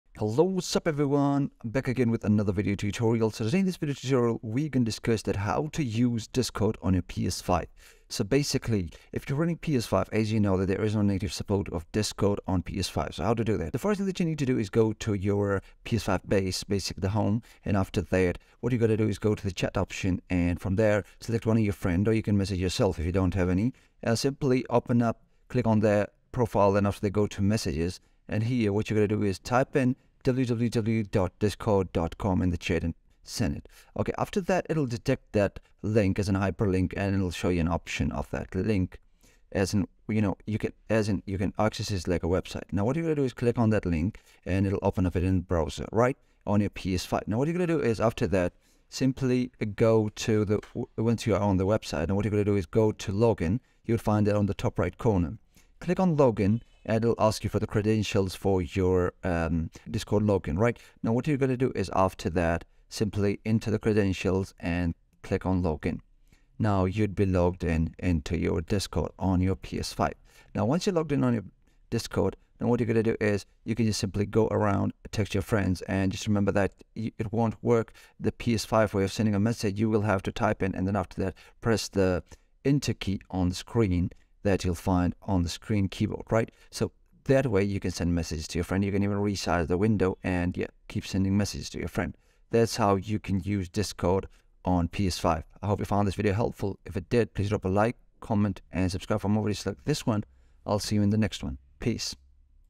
How To Use Discord On PS5 in 2023
Learn How To Use Discord On PS5 in 2023. With this tutorial it should be really easy and learnable within a few minutes.